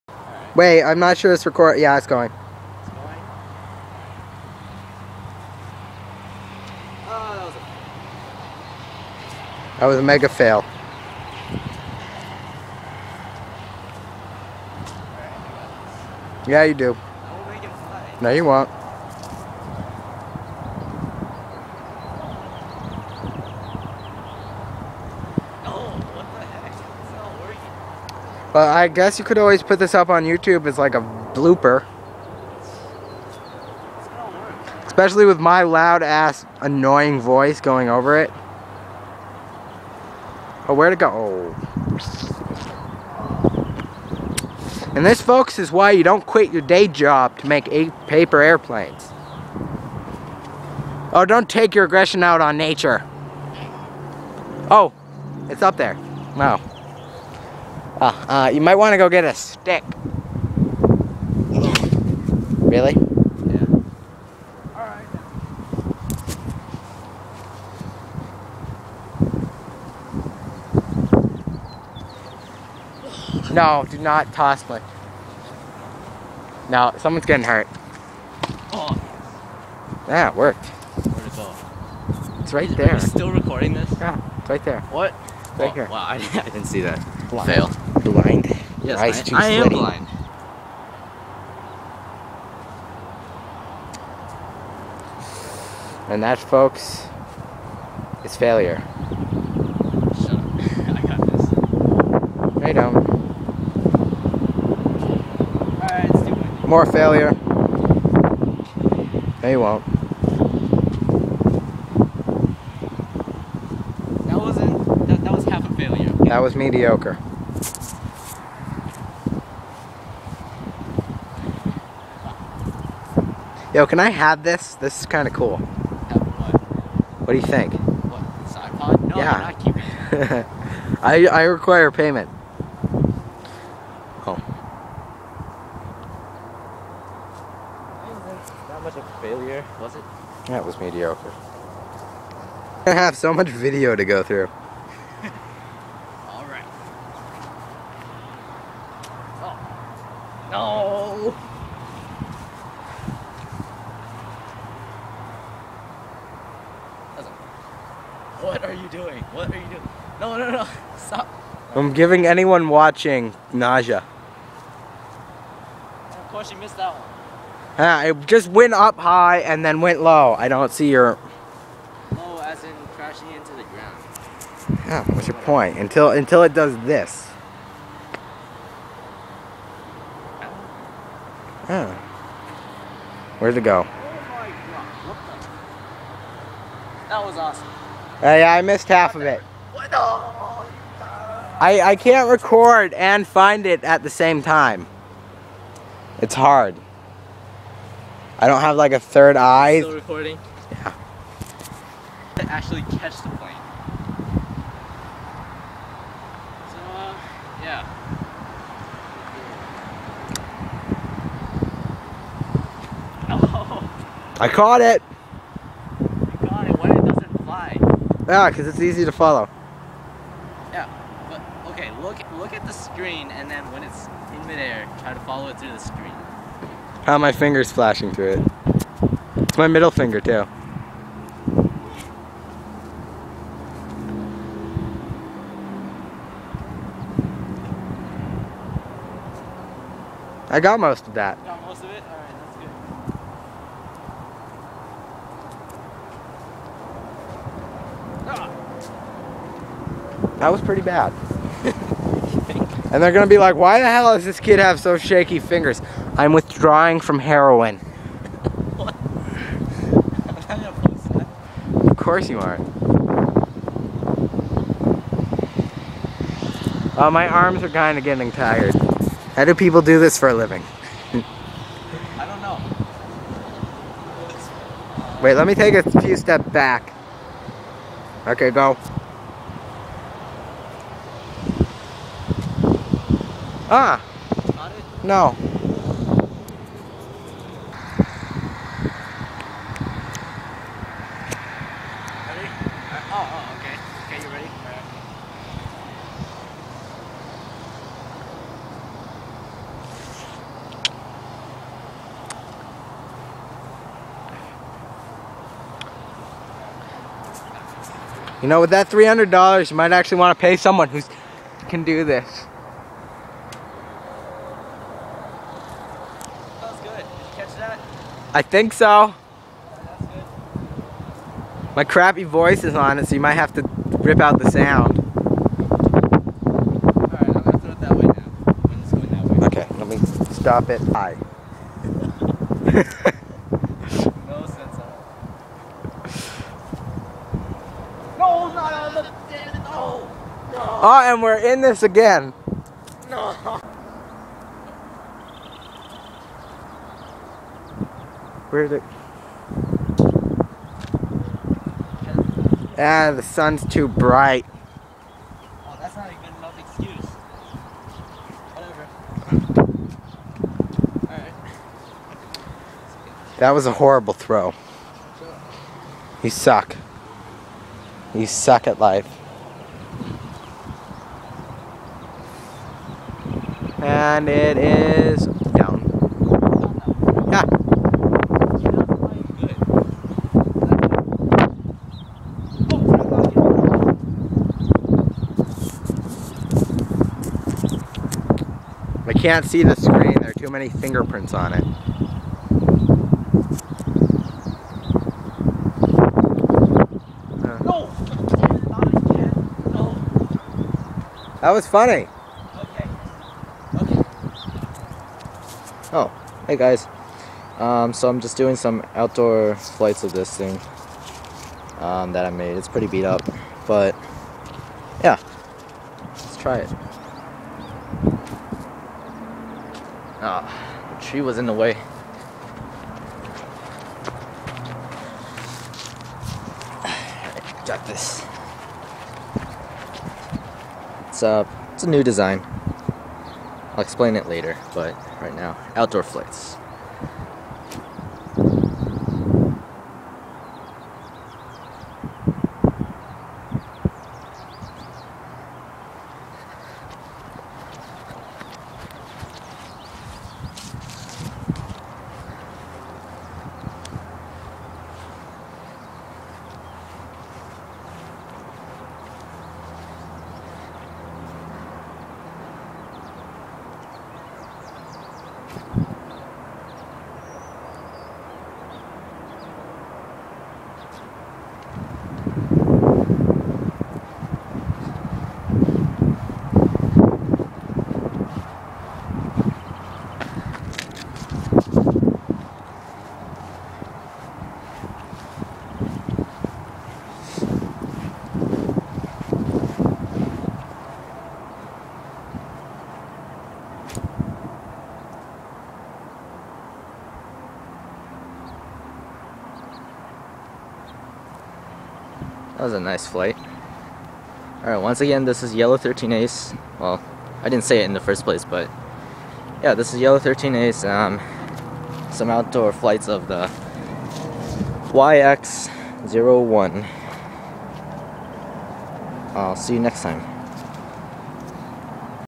Paper YX-01 "NightBat" Prototype #1 Outdoors Test Flight
My friend and I decided to shoot this video a while back (don't mind his "crappy voice" or his jokes). We were being silly, but we also flight-tested the very first, crude YX-01 prototype, which flew pretty well. The refined, aesthetic based finalized prototype did not do so well; it had pitch issues.

~yellow13ace